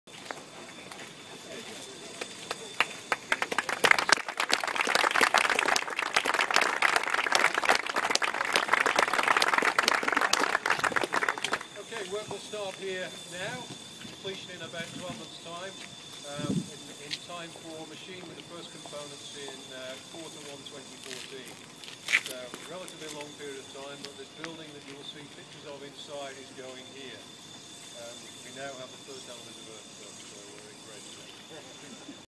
Site works begin for Australia's biggest titanium facility
Adelaide, Australia: Site works for Australia's biggest titanium manufacturing facility started today in Adelaide.

South Australian Premier Jay Weatherill turned the first sod of dirt on site at a ground breaking event held at BAE Systems Edinburgh Park site.

A collaboration between the State Government and BAE Systems, this project will ensure that South Australia has the advanced manufacturing skills required to supply long thin titanium components for the Joint Strike Fighter program.

BAE Systems Chief Executive David Allott said the investment would deliver long-term benefits.

"It reinforces the South Australian Government's long-term commitment for our state to become an advanced manufacturing leader with an important role in the F-35 supply chain over the next 20 years," he said.

"This new facility will allow us to address a significant capability gap in advanced manufacturing in Australia," he said.

"Also, the investment may lead to new opportunities in defence and other sectors like commercial aviation."

Site works are expected to be finished late July 2013, with manufacturing of the long thin Joint Strike Fighter components expected to begin early in 2014.